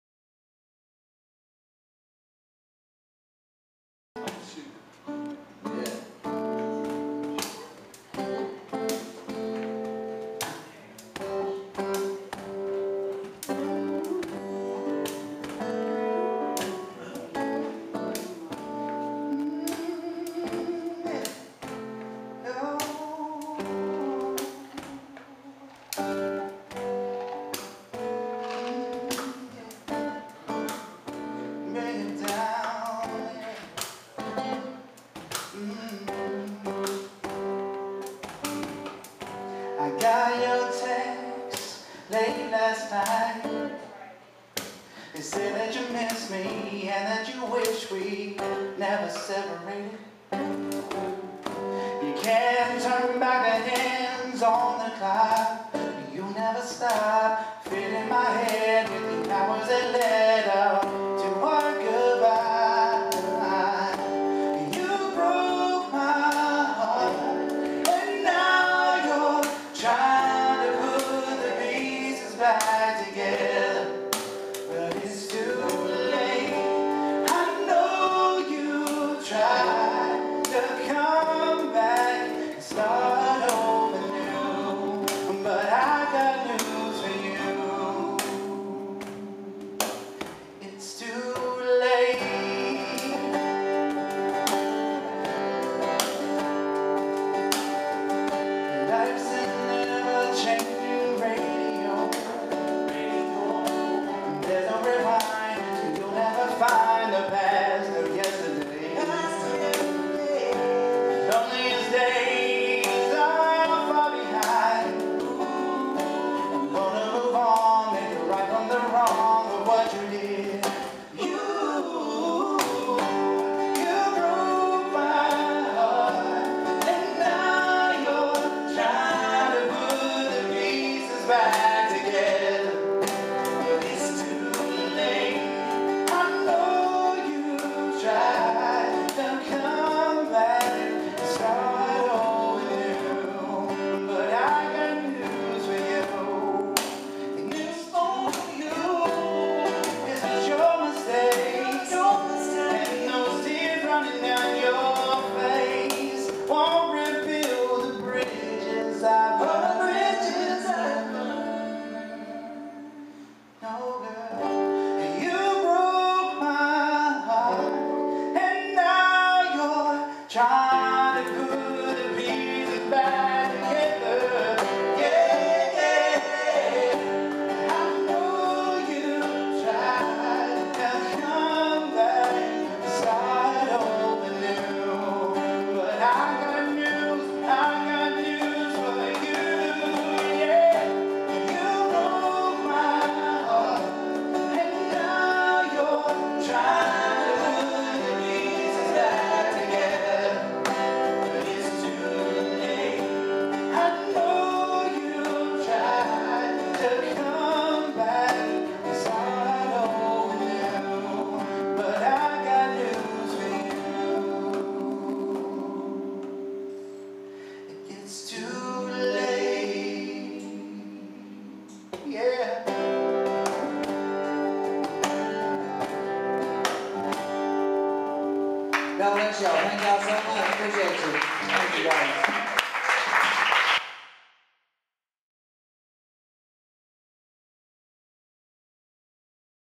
"The Trio" jammin at the Pavilion Coffee House.
Three guys having fun harmonizing together and entertaining the Pavilion Coffee House. This is a good example of pratice and talent coming together to make a really nice musical experience. Good music!